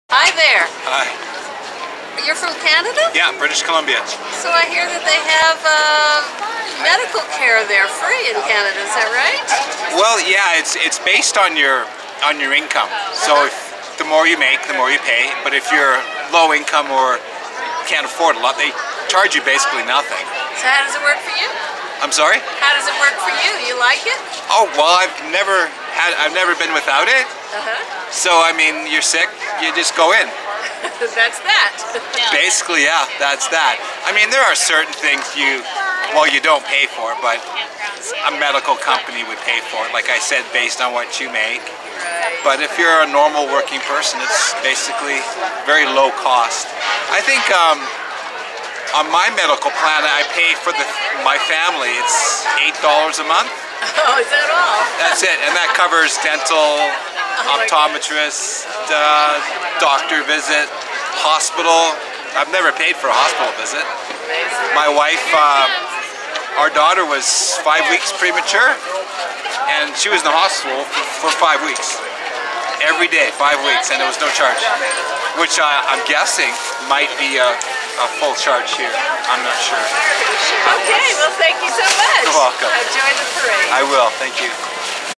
Health Care: July 4, 2008 Salmon Vistor Describes
At the Independence Day Parade in Salmon, Idaho, a British Columbia, Canada citizen describes health care in Canada.